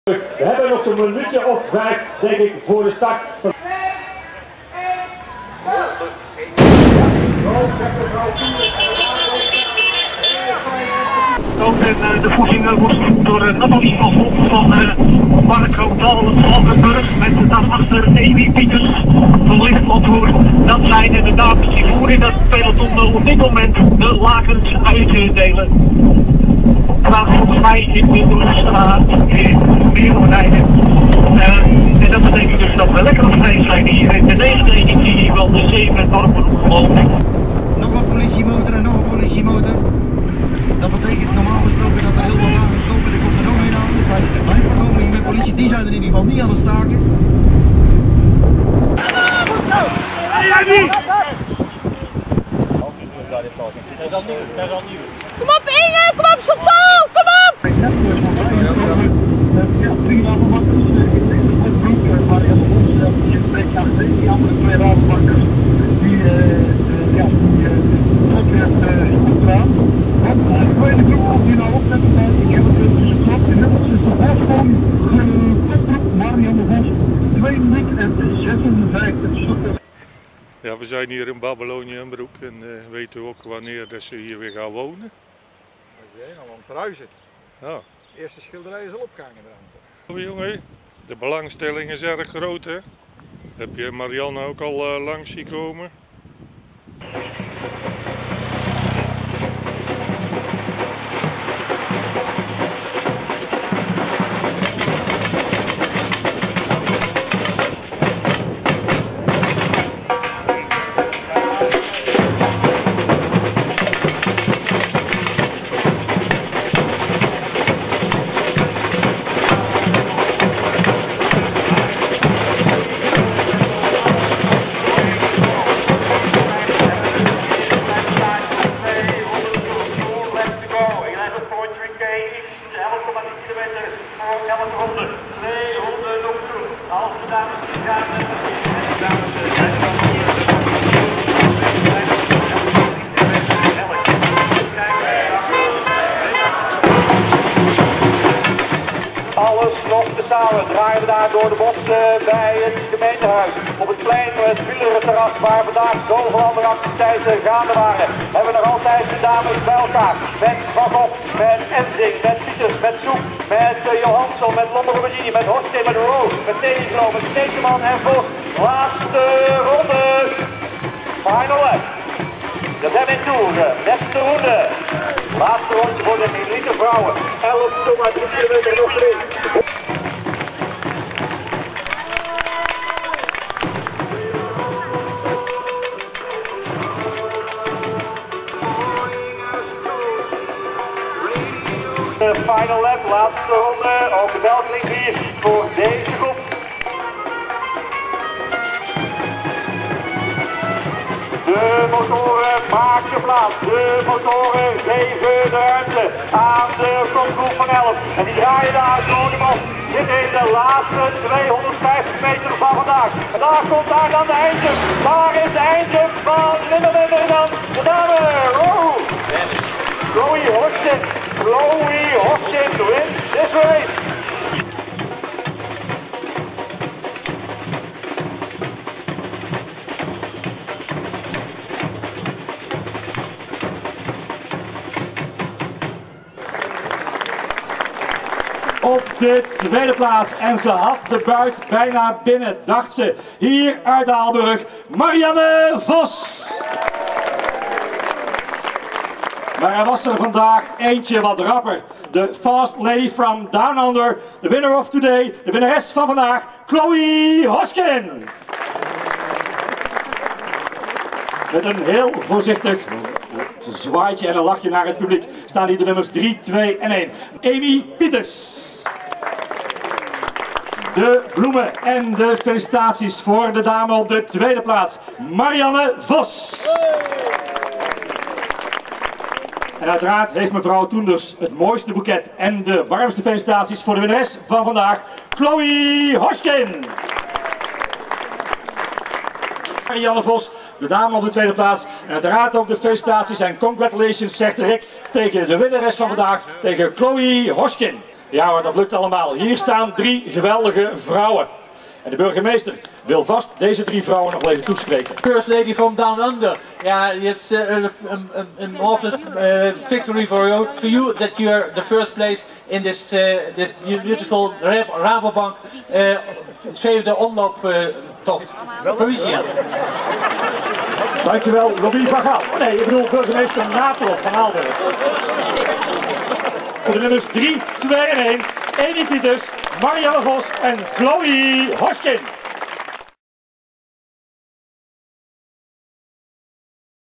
7 dorpenomloop Aalburg 9 mei 2015
Chloe Hosking heeft de 7-dorpenomloop Aalburg 2015 gewonnen. Marianne Vos eindigde met een gebroken rib tweede, de derde plaats was voor Amy Pieters.